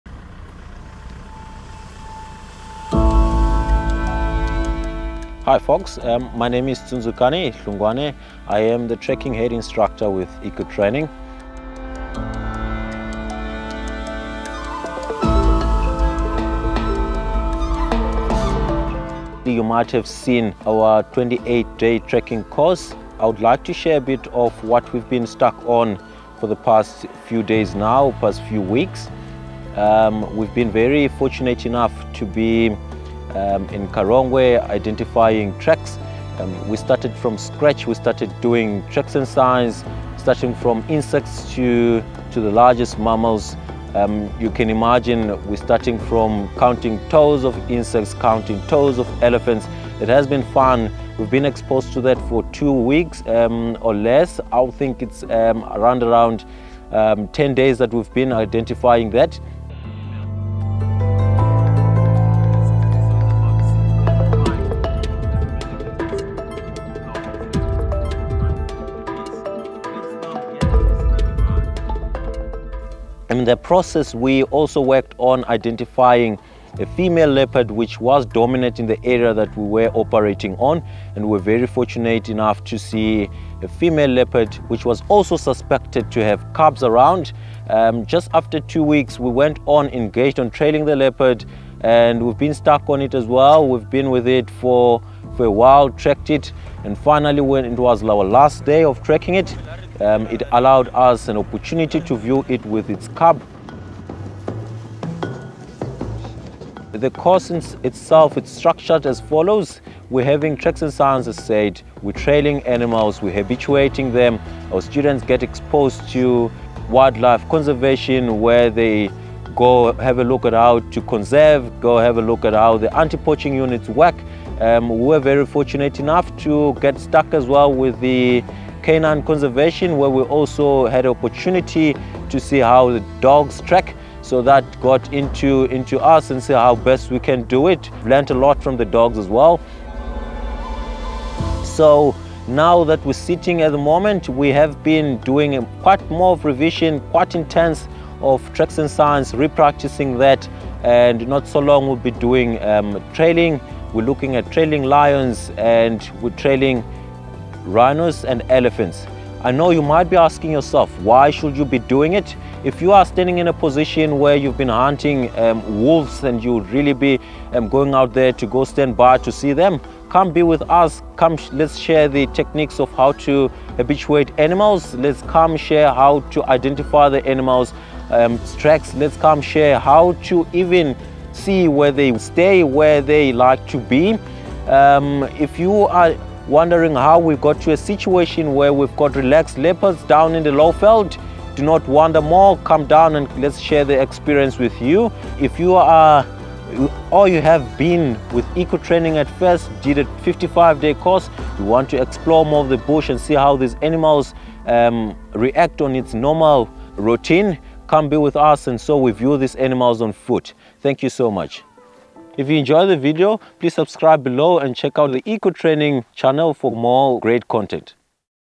28 day EcoTracker Course: Animal Monitoring Course | EcoTraining
A first-of-its-kind tracking course in South Africa! 🐾

Today we join EcoTraining Instructor Tsundzukani Hlongwane in Karongwe, where we join the current 28-day EcoTracker students. They give us a glimpse into their experiences while on the course. One incredible experience was tracking a female leopard with her cub. What an excellent opportunity!

On the 28-day EcoTracker course, you will participate in tracking projects: Phase one focuses on developing your tracking knowledge and skills. In phase two, you will learn how to apply wildlife tracking practically by being exposed to predator monitoring and wildlife protection. You will embark on an exciting tracking project where you will learn from specialist instructors. In phase three, you will have the opportunity to be evaluated on an FGASA-endorsed Tracker Academy practical tracking assessment.